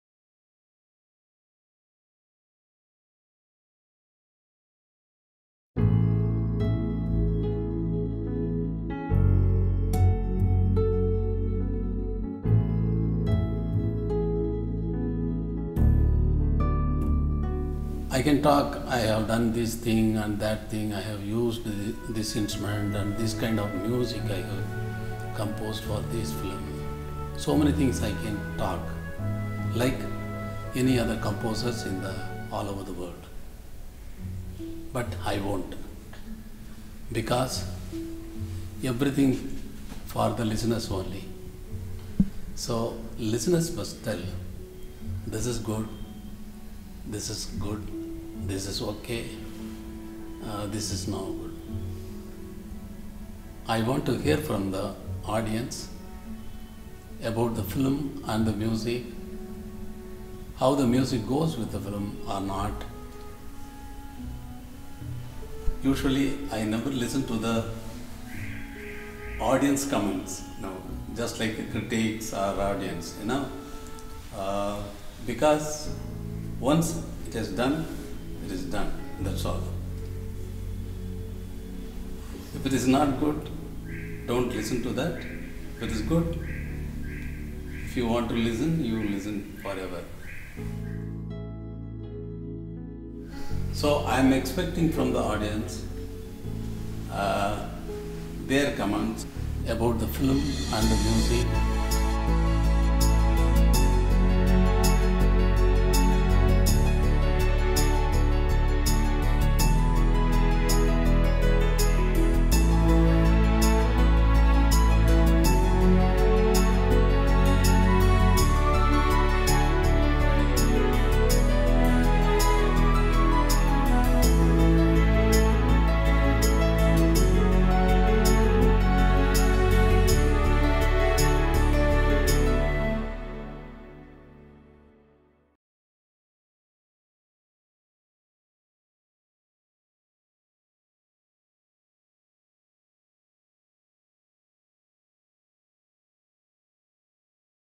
LOVE AND LOVE ONLY | Special Message From Ilaiyaraaja
See 'Love And Love Only' in Cinemas with FanForce NOW!

This is the first English film for which India's legendary composer Ilaiyaraaja has composed original songs and score.

Synopsis:When a rich Indian International student falls for a working class Australian girl, who is crazily in Love with him, how will he handle the cultural differences and succeed in his love, against his parents' plans for his future?